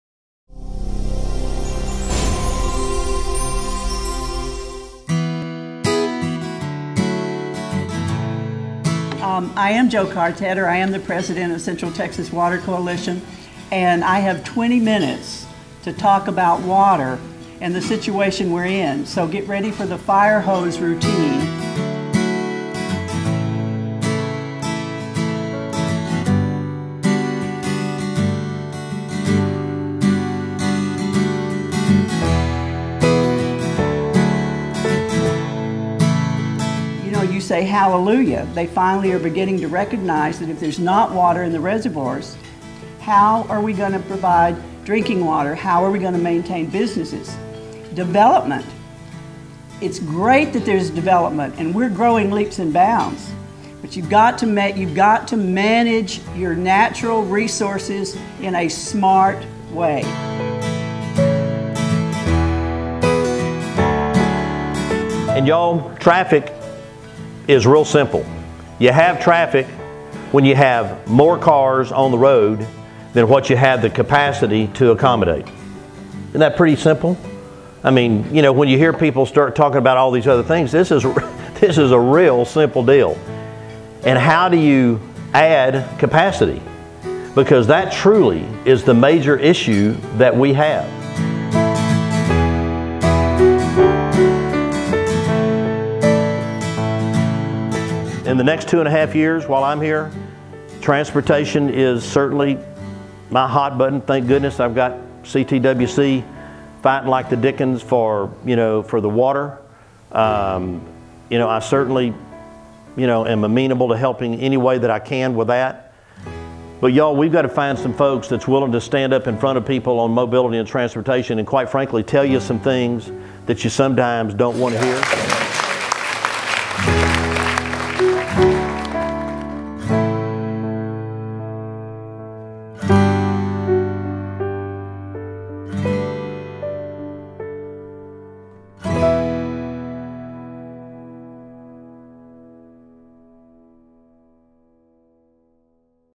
Lake Travis Chamber: "Transportation & Water" highlight reel
lake travis chamber of commerce may luncheon - transportation & water

PRESENTERS:  Jo Karr Tedder, President of the Central Texas Water Coalition.

• Former Educator/Administrator, Educational Consultant, and Political Organizer
• Texas native; Returned to Texas from East Coast; Lived in Burnet County since 2001
• Member of Andrew Sansom's Grand Water Challenge Group; Central TX Lakes, Rivers and Bays Group; Burnet County Comprehensive Planning Committee, Burnet County Transportation Committee, Commissioner for ESD #5 (East Lake Buchanan), Past Chair of Burnet Bluebonnet Festival; and Past President of Council Creek Village Property Owners Association
• President of Central Texas Water Coalition, Inc. after serving on LCRA's Water Management Plan as a representative of Lake Buchanan

Precinct Three Commissioner Gerald Daugherty, Travis County

Gerald Daugherty is a 1975 graduate of the University of Texas. He has been an Austin businessman for more than 28 years; owning and operating several successful ventures, his most recent being Jack Allen's Kitchen...a very popular restaurant located in Oak Hill and the newest Jack Allen's Kitchen at 2500 Hoppe Trail in Round Rock.

Gerald was elected to the Travis County Commissioners Court in November of 2002 where he served as commissioner of Precinct 3 until 2008. He is a true fiscal conservative. His main concern then was to keep county taxes at bay and he backed up his words when he took a 10% cut in his salary each of the six years he served. He still believes there are ways to curb the county's spending and will continue to be the lone voice of conservatism on the court.

A long-time advocate of a comprehensive transportation plan for Austin and Travis County, Gerald is committed to getting SH 45 SW built and to working toward a solution to all our traffic problems.

Commissioner Daugherty is considered by many on both sides of the political spectrum as a listener and problem solver. He is concerned about the future of Travis County and for that reason has chosen to once again seek a seat on the Commissioners' Court.